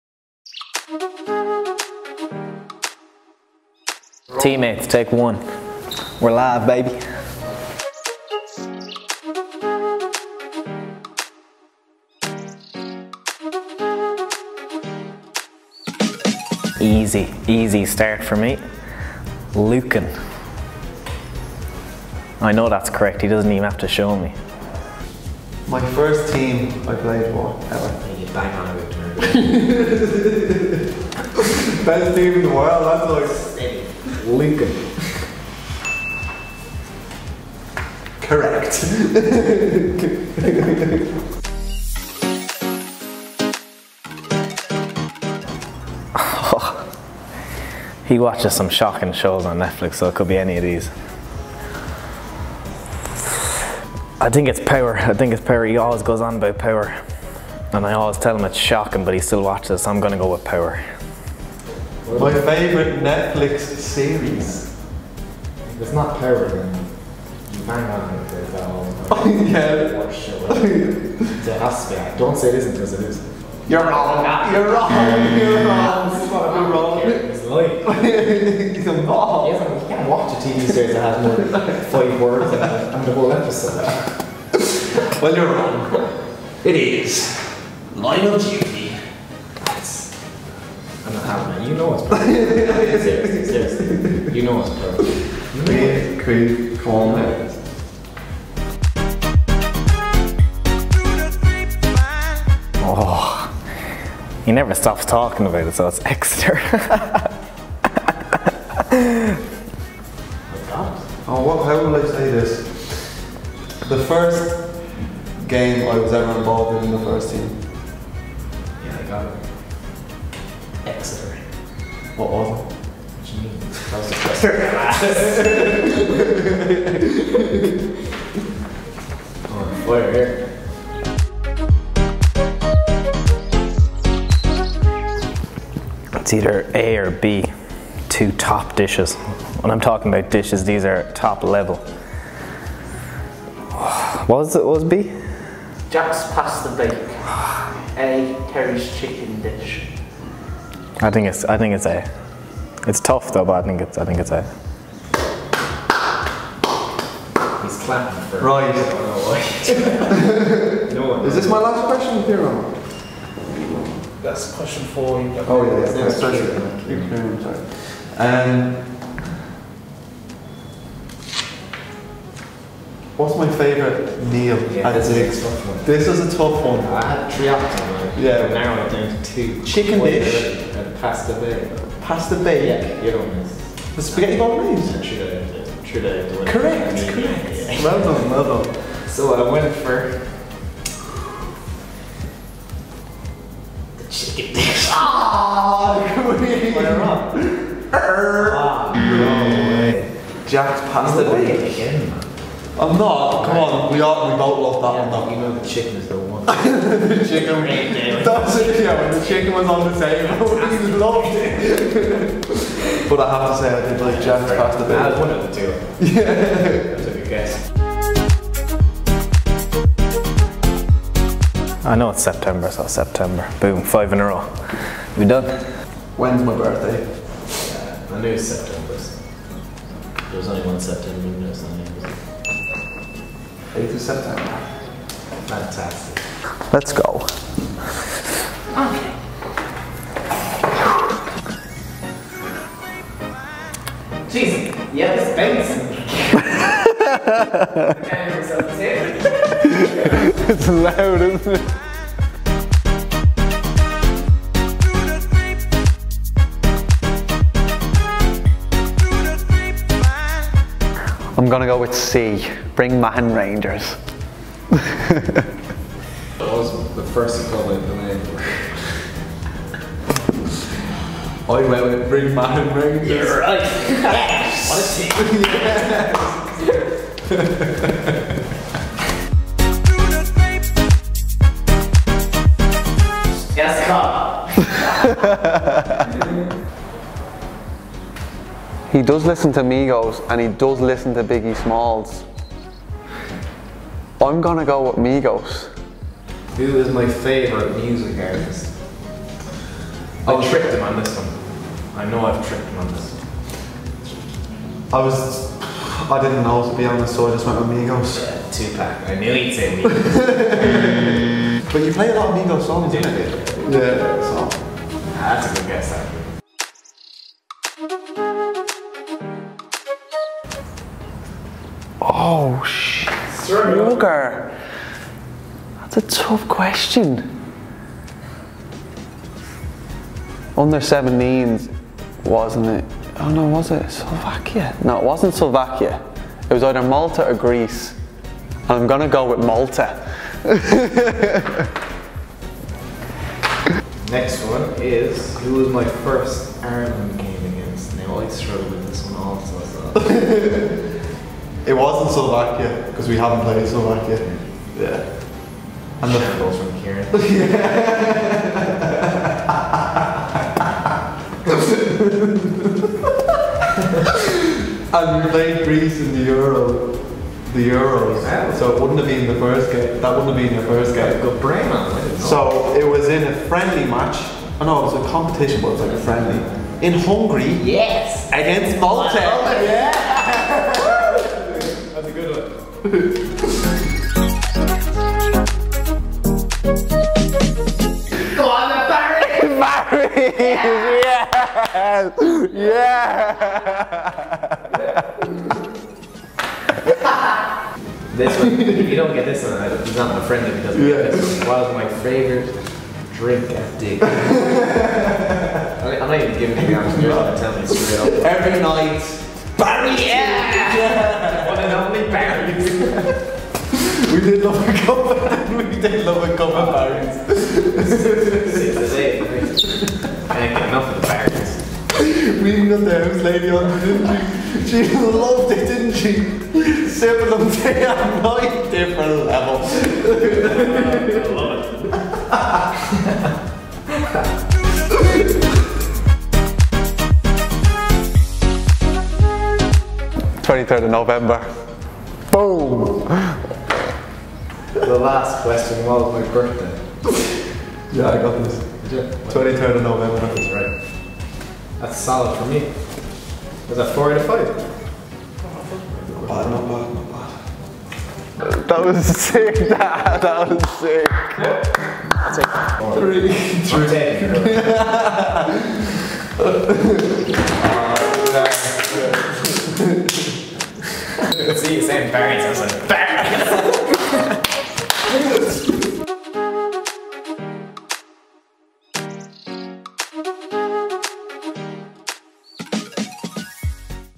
IRLU21 Roommates | Caoihmin Kelleher & Conor Masterson
Republic of Ireland U-21 internationals Caoimhin Kelleher and Conor Masterson test each other at Roommates, a must watch!